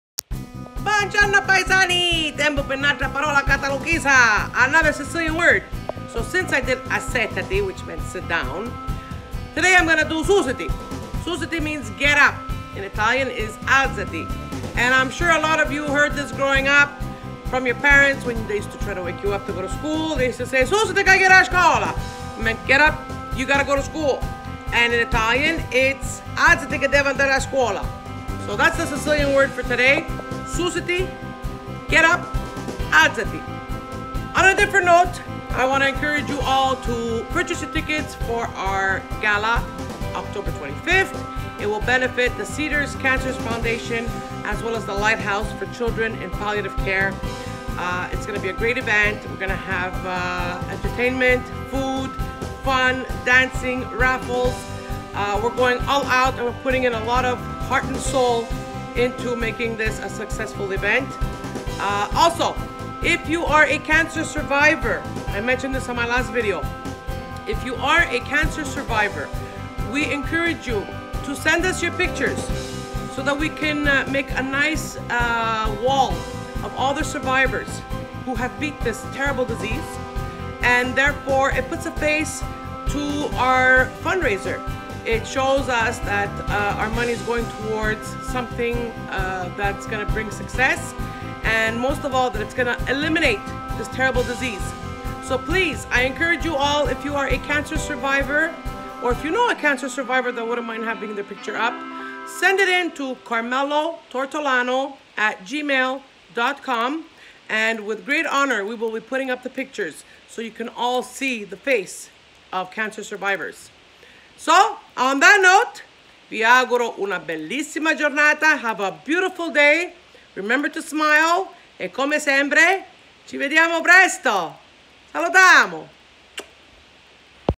Sicilian is a language
Today’s sicilian word is “susiti” means get up and in Italian it’s “alzati”
Salutamu